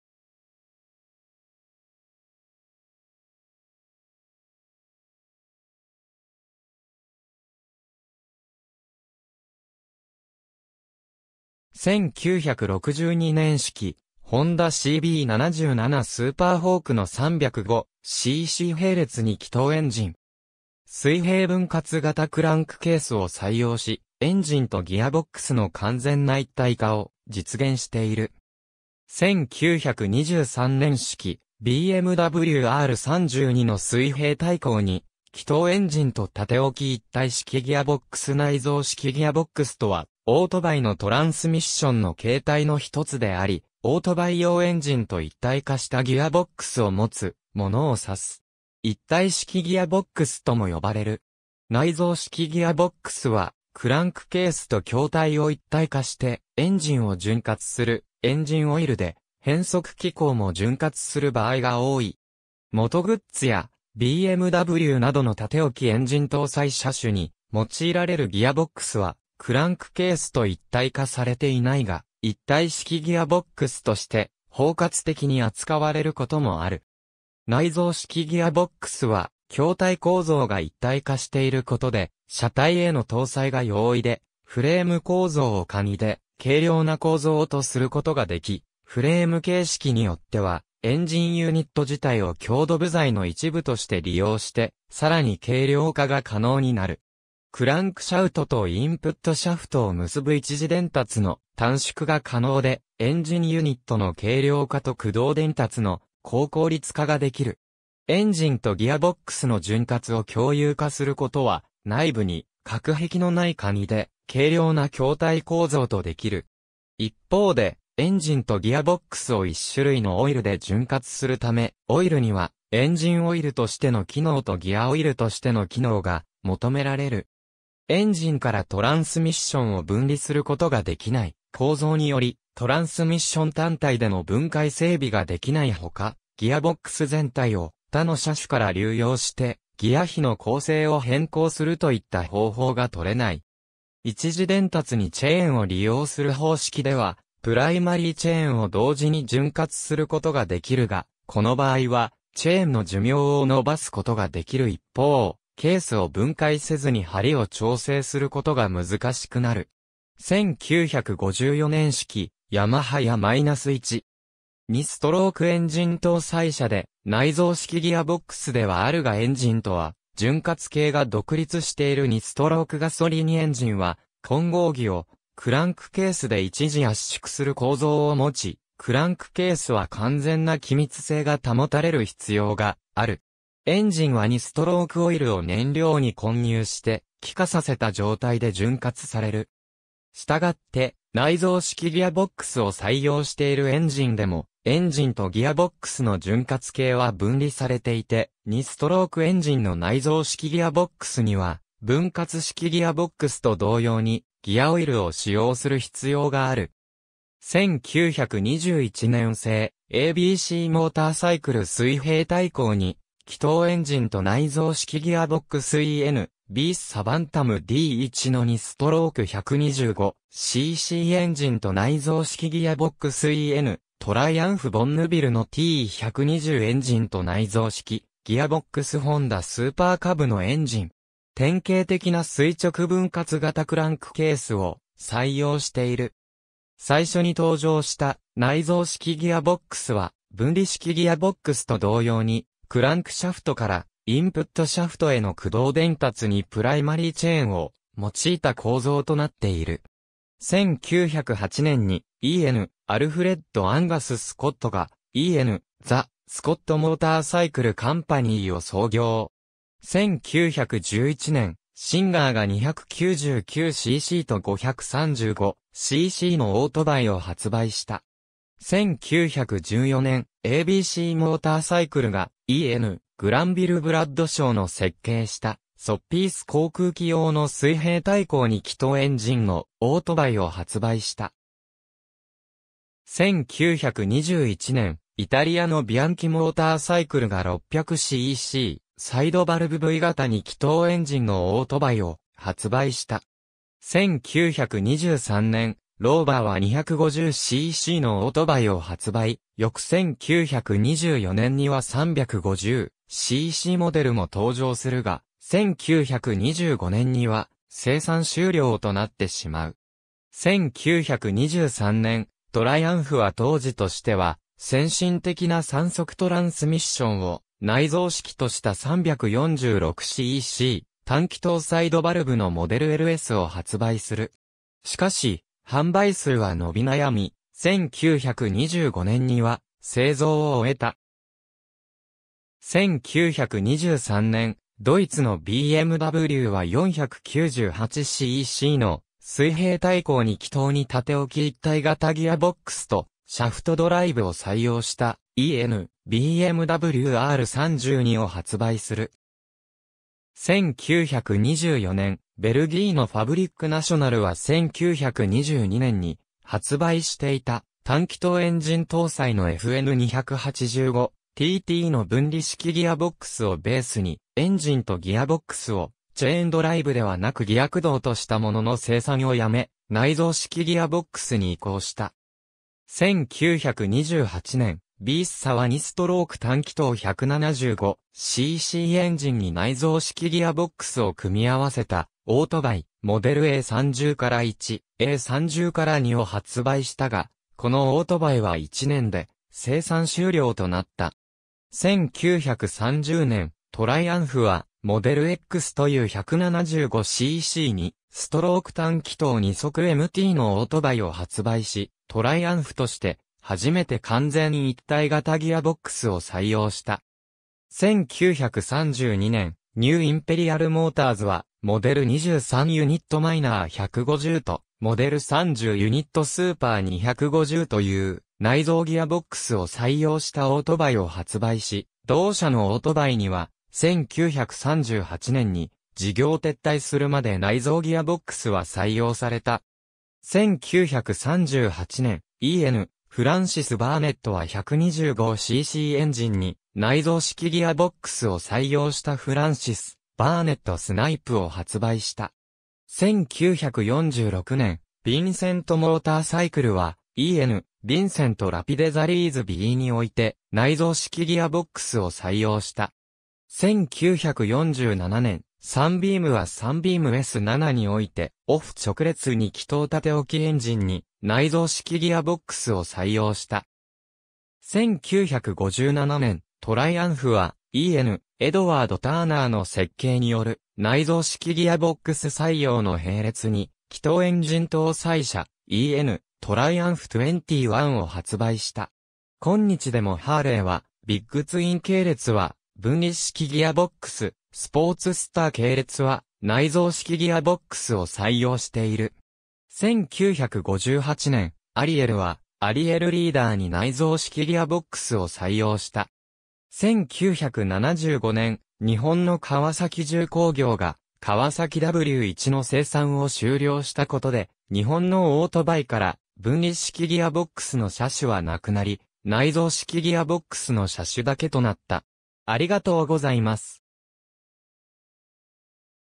内蔵式ギアボックス
1962年式ホンダ・CB77 スーパーホークの305cc並列2気筒エンジン。
水平分割型クランクケースを採用し、エンジンとギアボックスの完全な一体化を実現している。
1923年式BMW・R32の水平対向2気筒エンジンと縦置き一体式ギアボックス 内蔵式ギアボックス（ないぞうしきギアボックス）とは、オートバイのトランスミッションの形態の一つであり、オートバイ用エンジンと一体化したギアボックスを持つものを指す。
一体式ギアボックス(Unit construction)とも呼ばれる。
内蔵式ギアボックスはクランクケースと筐体を一体化して、エンジンを潤滑するエンジンオイルで変速機構も潤滑する場合が多い。
モト・グッツィやBMWなどの縦置きエンジン搭載車種に用いられるギアボックスはクランクケースと一体化されていないが、一体式ギアボックスとして包括的に扱われることもある。
内蔵式ギアボックスは筐体構造が一体化していることで、車体への搭載が容易でフレーム構造を簡易で軽量な構造とすることができ、フレーム形式によってはエンジンユニット自体を強度部材の一部として利用して、さらに軽量化が可能になる。
クランクシャウトとインプットシャフトを結ぶ一次伝達の短縮が可能で、エンジンユニットの軽量化と駆動伝達の高効率化ができる。
エンジンとギアボックスの潤滑を共有化することは、内部に隔壁のない簡易で軽量な筐体構造とできる。
一方で、エンジンとギアボックスを一種類のオイルで潤滑するため、オイルにはエンジンオイルとしての機能とギアオイルとしての機能が求められる。
エンジンからトランスミッションを分離することができない構造により、トランスミッション単体での分解整備ができないほか、ギアボックス全体を他の車種から流用してギヤ比の構成を変更するといった方法が採れない。
一次伝達にチェーンを利用する方式ではプライマリーチェーンを同時に潤滑することができるが、この場合はチェーンの寿命を延ばすことができる一方、ケースを分解せずに張りを調整することが難しくなる。
1954年式ヤマハ・YA-1。
2ストロークエンジン搭載車で、内蔵式ギアボックスではあるがエンジンとは潤滑系が独立している 2ストロークガソリンエンジンは混合気をクランクケースで一次圧縮する構造を持ち、クランクケースは完全な気密性が保たれる必要がある。
エンジンは2ストロークオイルを燃料に混入して気化させた状態で潤滑される。
したがって、内蔵式ギアボックスを採用しているエンジンでも、エンジンとギアボックスの潤滑系は分離されていて、2ストロークエンジンの内蔵式ギアボックスには分割式ギアボックスと同様にギアオイルを使用する必要がある。
1921年製ABCモーターサイクル水平対向2気筒エンジンと内蔵式ギアボックス en:BSA Bantam D1の2ストローク125ccエンジンと内蔵式ギアボックス en:Triumph BonnevilleのT120エンジンと内蔵式ギアボックス ホンダ・スーパーカブのエンジン。
典型的な垂直分割型クランクケースを採用している。
最初に登場した内蔵式ギアボックスは分離式ギアボックスと同様に、クランクシャフトからインプットシャフトへの駆動伝達にプライマリーチェーンを用いた構造となっている。
1908年にen:Alfred_Angas_Scottがen:The Scott Motorcycle Companyを創業。
1911年、Singerが299ccと535ccのオートバイを発売した。
1914年、ABCモーターサイクルがen:Granville Bradshawの設計したソッピース航空機用の水平対向2気筒エンジンのオートバイを発売した。
1921年、イタリアのBianchiモーターサイクルが600ccサイドバルブV型2気筒エンジンのオートバイを発売した。
1923年、ローバーは250ccのオートバイを発売、翌1924年には350ccモデルも登場するが、1925年には生産終了となってしまう。
1923年、トライアンフは当時としては先進的な3速トランスミッションを内蔵式とした346cc単気筒サイドバルブのModel LSを発売する。
しかし販売数は伸び悩み、1925年には製造を終えた。
1923年、ドイツのBMWは498ccの水平対向2気筒に縦置き一体型ギアボックスとシャフトドライブを採用したen:BMW_R32を発売する。
1924年、ベルギーのファブリック・ナショナルは1922年に発売し...